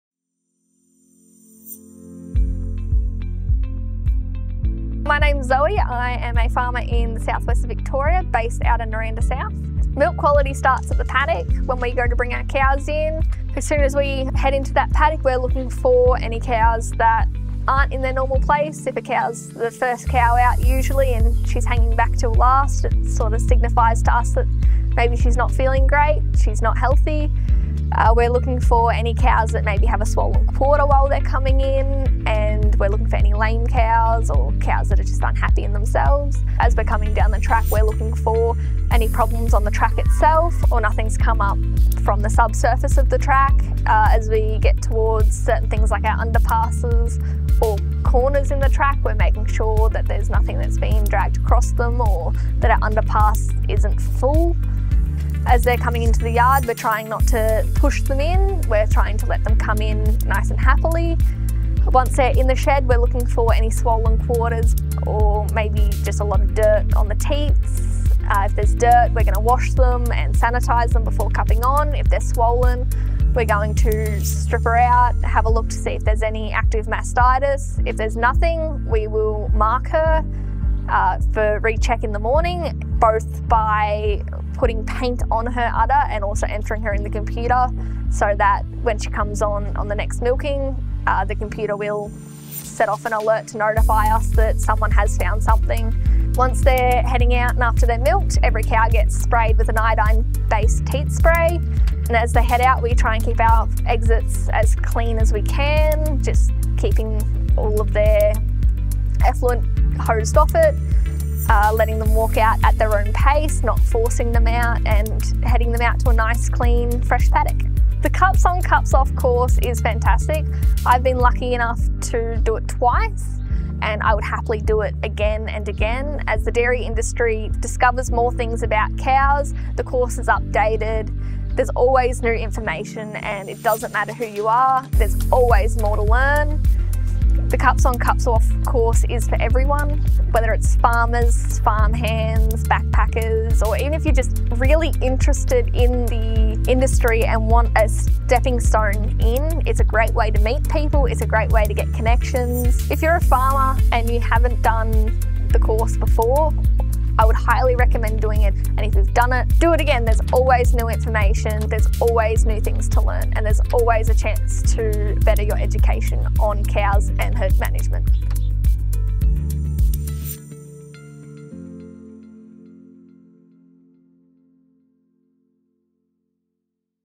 Milking and Mastitis Management
Hear from Zoe Greenslade, a dairy farmer from south west Victoria, about her experience and know-how in mastitis management on-farm and the value of undertaking a dedicated workshop to stay up to date with industry developments and best practice management.